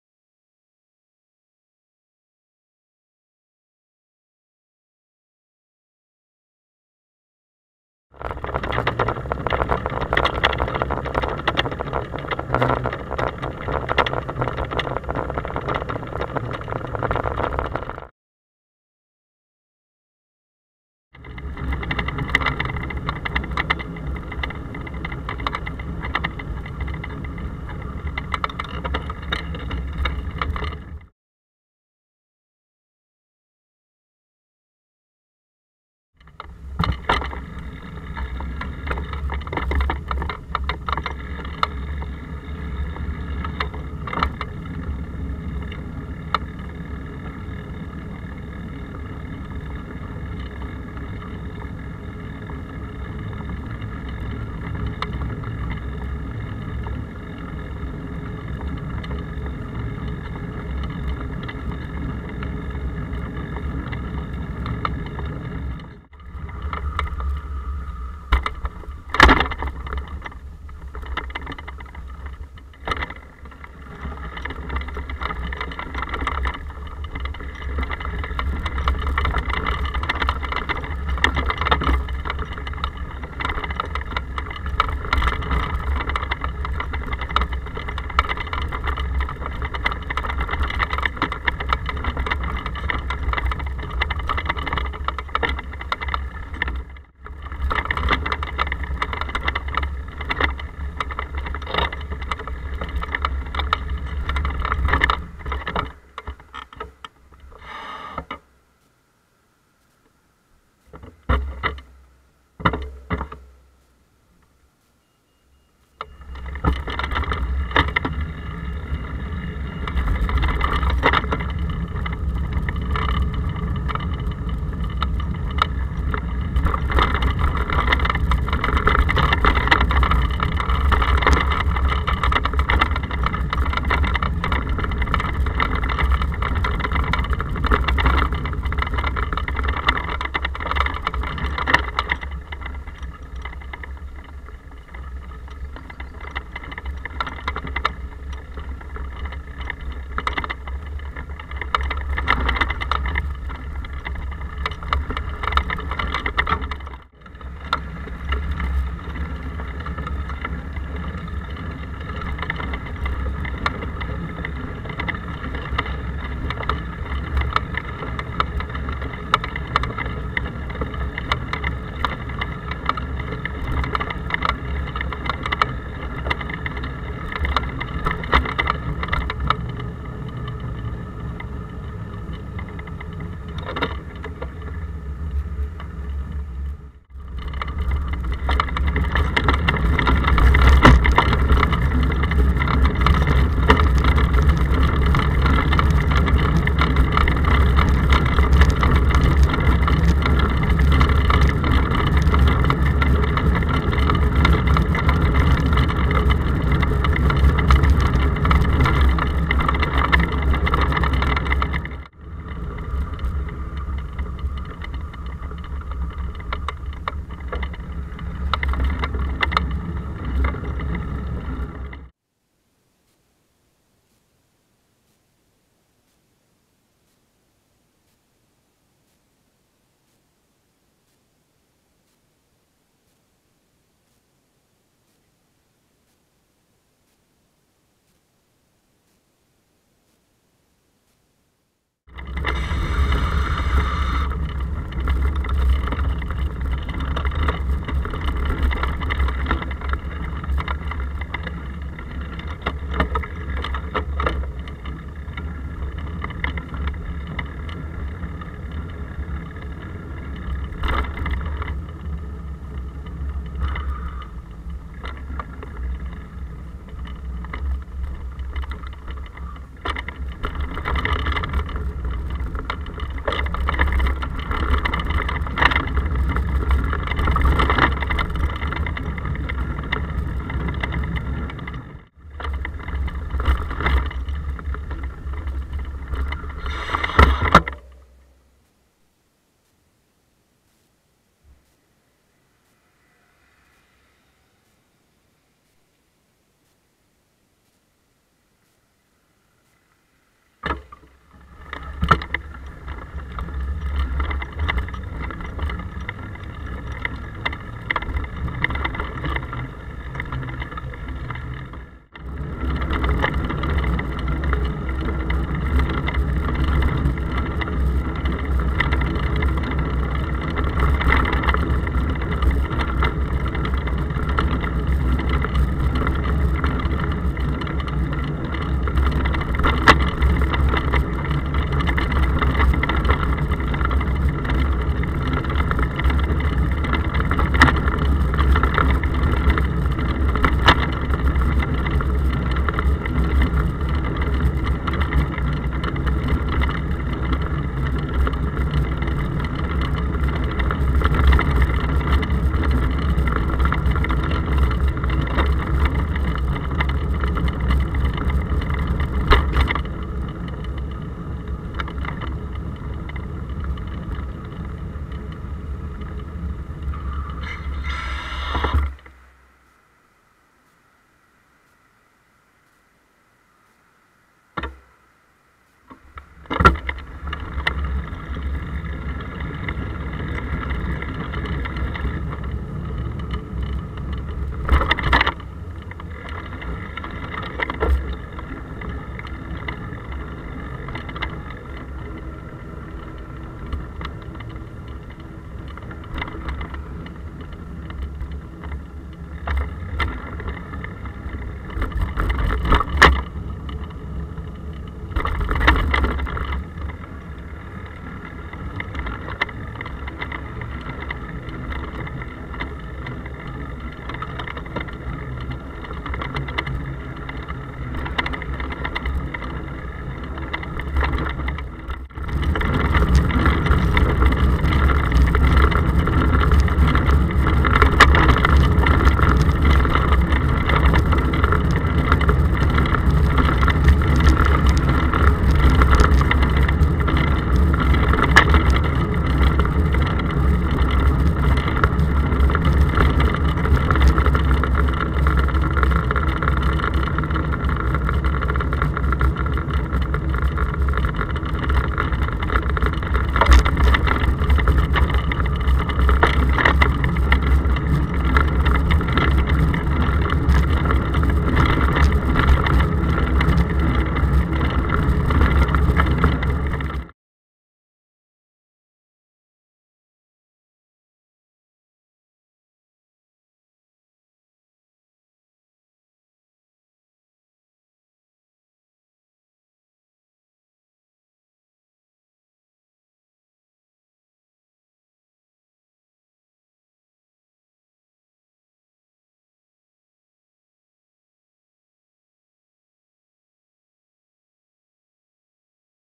Second test run of Apeman Action Camera
Second test run of Apeman Action Camera with added anti-vibration strip in the case, tested for night shot quality.